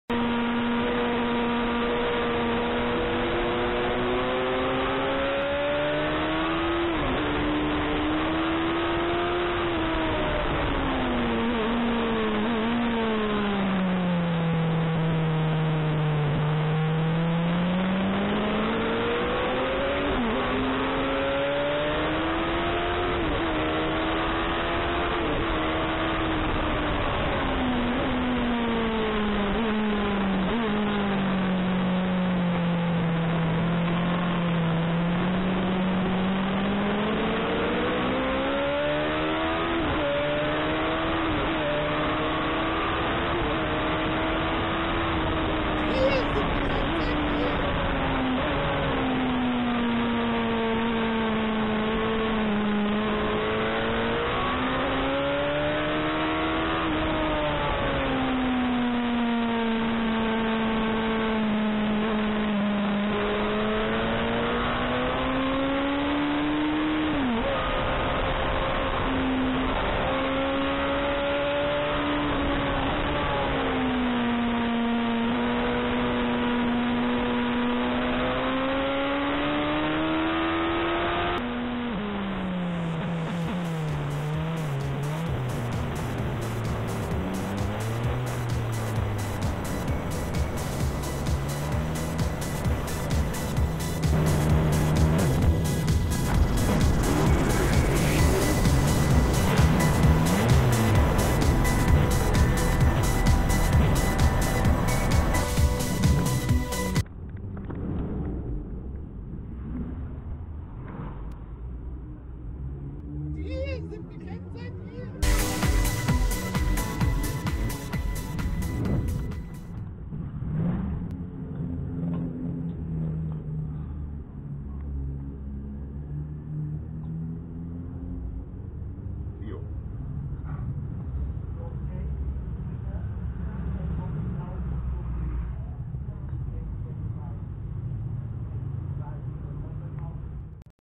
Wenn man nur an Bier denkt
Mit der Suzi bissl rumgecruist...und bei der RSV 4 an Bier gedacht...Prost Mike